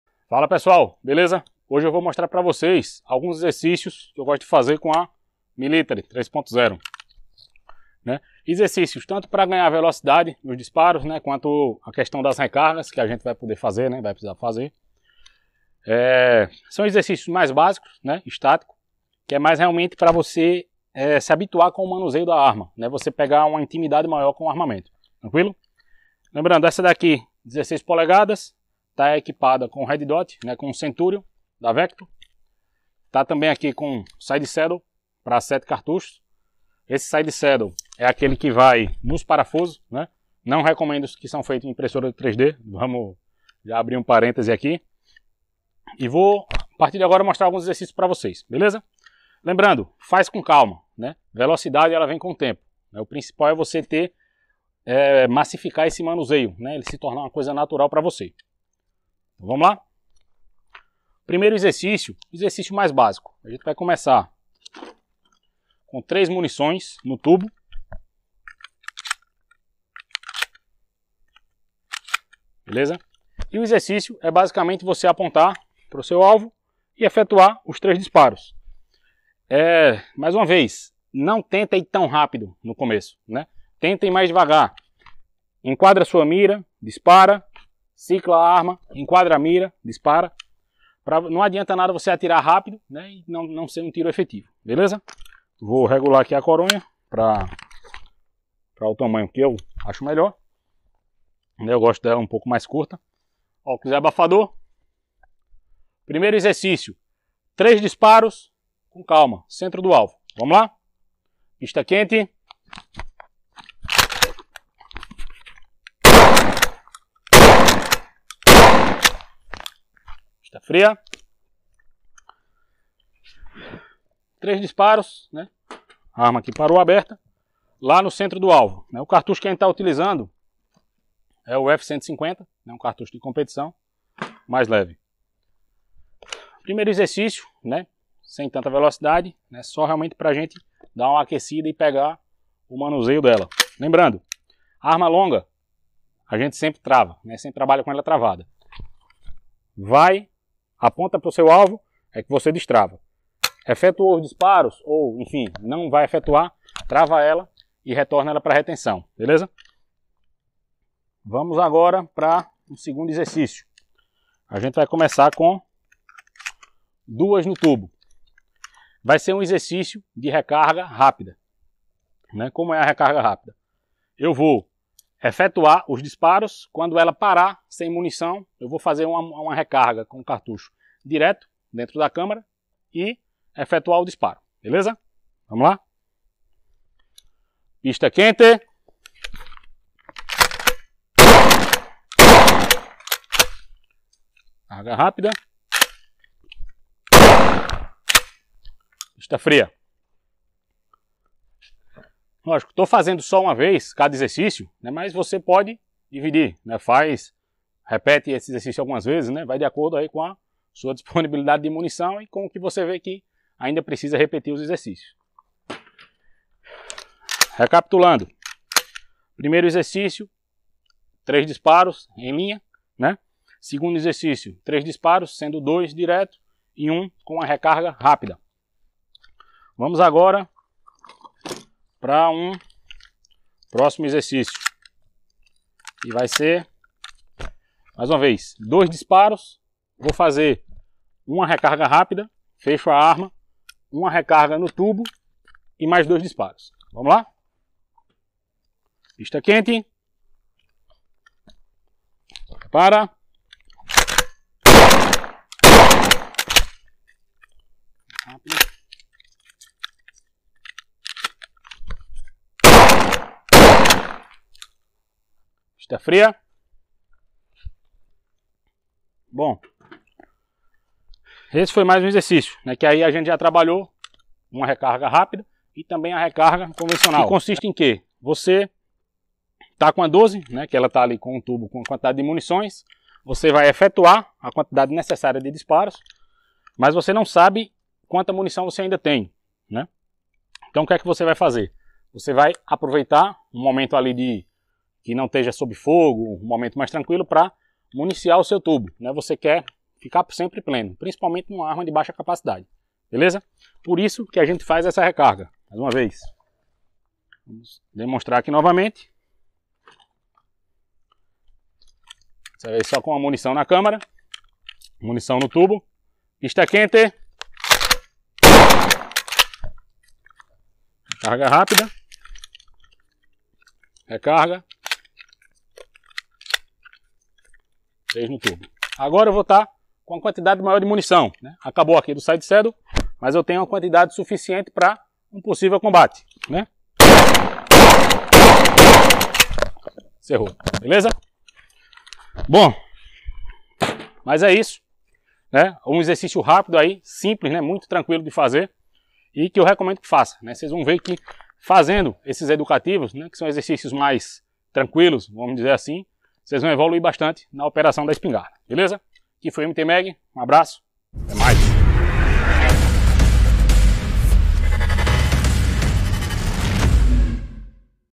Exercícios de Tiro com Espingarda Pump Military 3.0
O proposito do vídeo e passar alguns treinos básicos de tiro com a espingarda 12